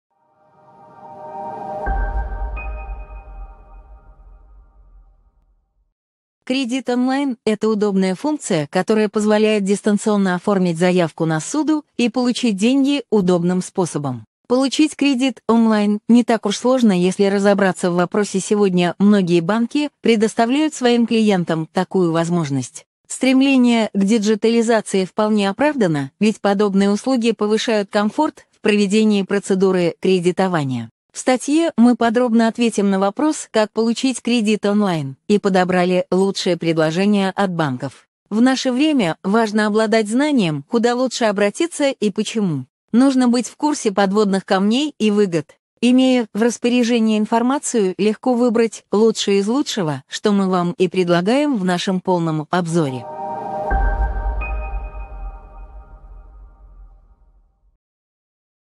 Что такое кредит онлайн и как его получить
Сегодня многие банки предоставляют своим клиентам оформить кредит онлайн. В статье мы подробно ответим на вопрос: «как получить кредит онлайн?» и подобрали лучшие предложения от банков. Подробнее